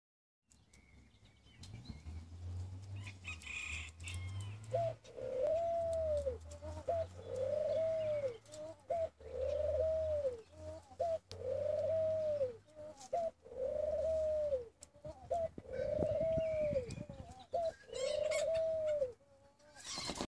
White turtle doves - mating ritual
The doves in our new aviary.  They are notorious for being overly amorous.